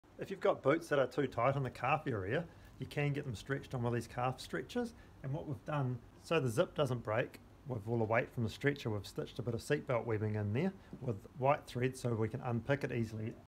Boots tight on your calf muscles? Get them on this boot stretcher!
Have you been going to the gym since you bought those lovely Italian leather boots? Now they kill your calves or you can't get the zip all the way up?

Introducing the boot calve stretcher. It's like a metal leg that goes in your boot. As you wind it splits in two and gets bigger, stretching your boot. Can be adjusted to widen more at the top, middle or ankle.